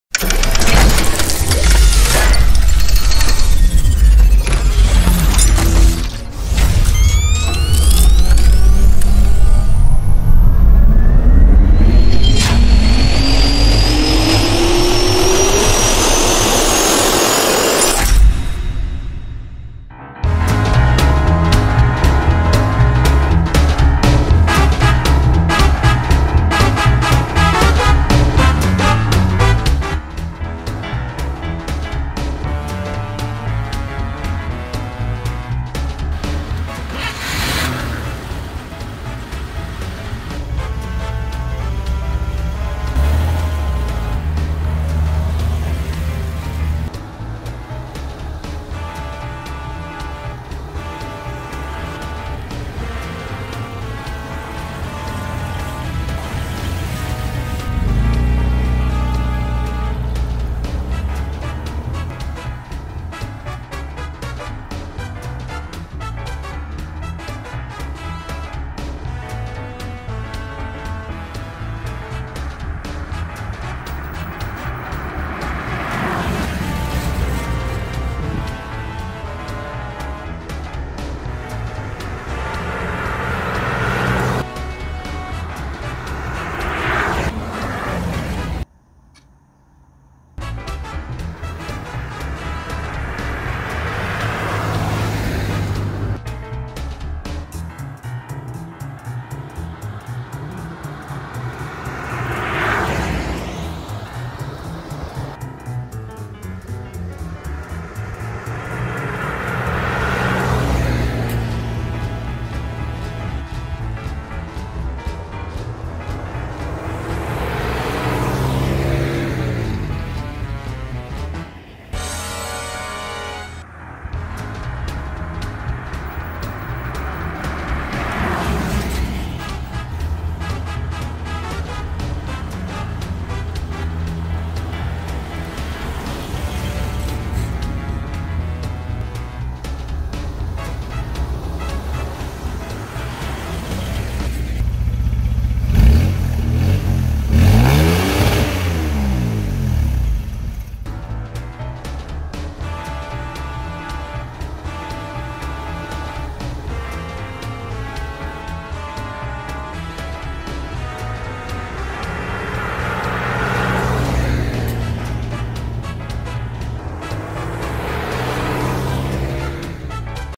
1939 Sharknose Graham - 1939 Graham Model 97 TV Commercial - Graham-Paige Motors Corporation
If the Graham Brothers had made a television commercial for the modern car enthusiast, what would it look like? This concept came from our imaginations. Watch as we apply some of today's technology to yesterday's innovation. We're sure that after seeing this ad, you'll fall in love with the Graham Sharknose!

Looking for Graham supercharger restoration?

Restoration and Driver: David Spence
Camera and Editing: Trish Spence
Camera Car: Sebastian Spence